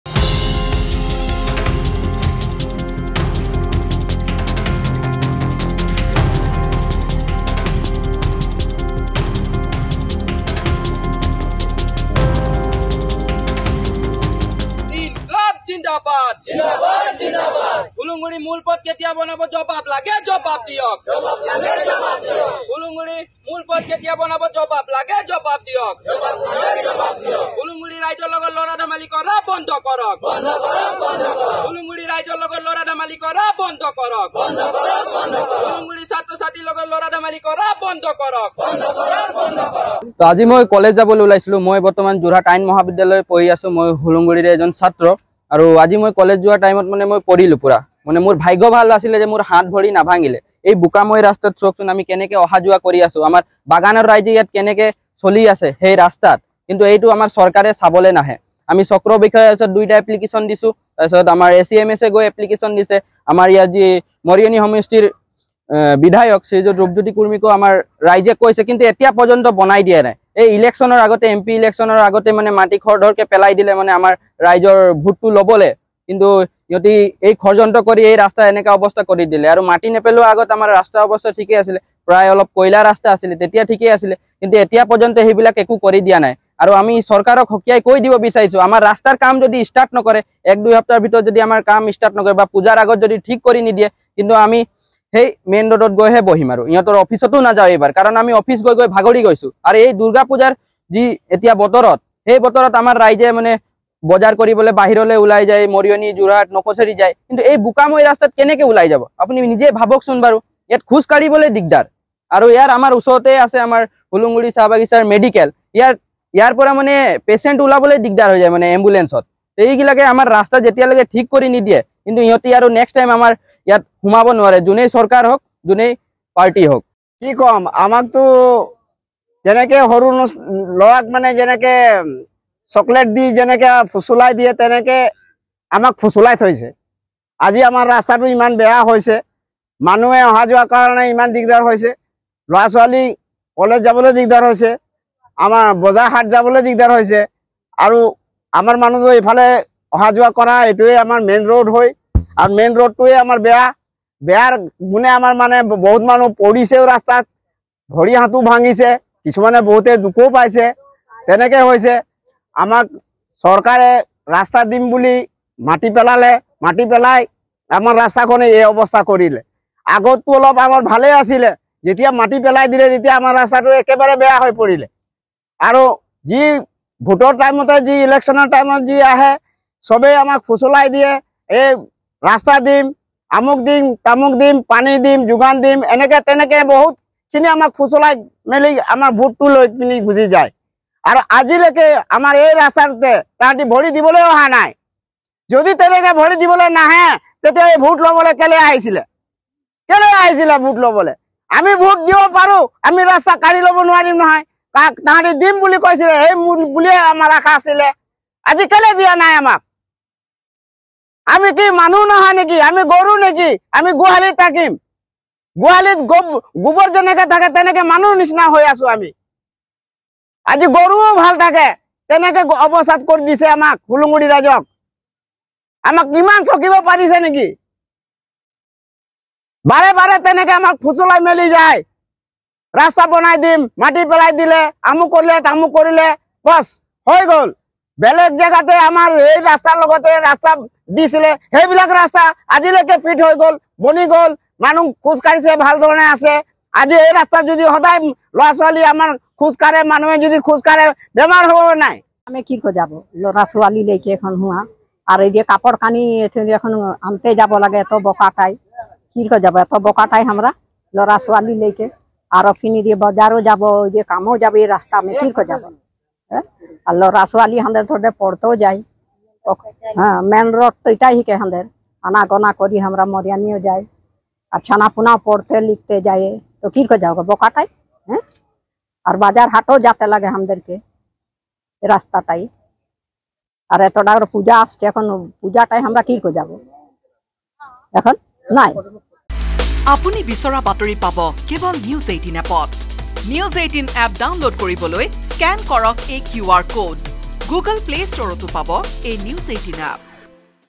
Mariani News : এটা পথে জ্বলা-কলা খুৱাইছে মৰিয়নিৰ হোলোংগুৰি চাহ বাগিচাৰ ৰাইজক | N18V
Mariani News | এটা পথে জ্বলা-কলা খুৱাইছে মৰিয়নিৰ হোলোংগুৰি চাহ বাগিচাৰ ৰাইজক